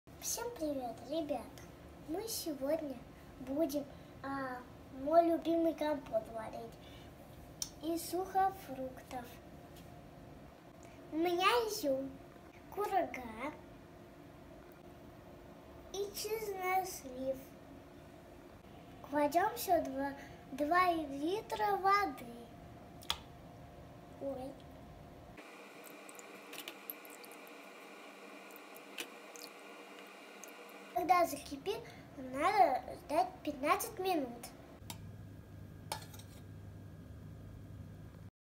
Компот из сухофруктов от мини шеф повара Алины.
С таким шеф поваром не пропадёшь! Готовит вкусно и полезно.